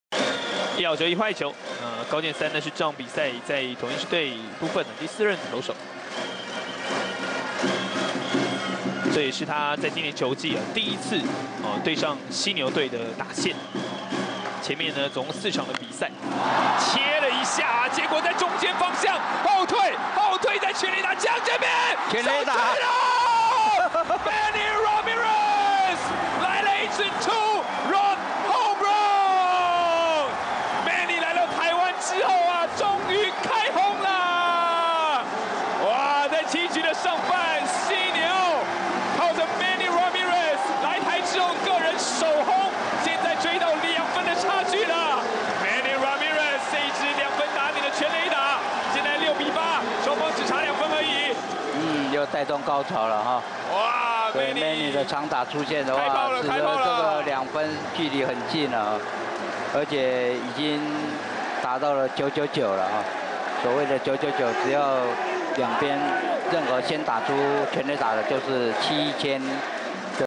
Manny Ramirez hits 1st home run in Taiwan & Announcer goes crazy
Manny Ramirez Playing OverSea in Taiwan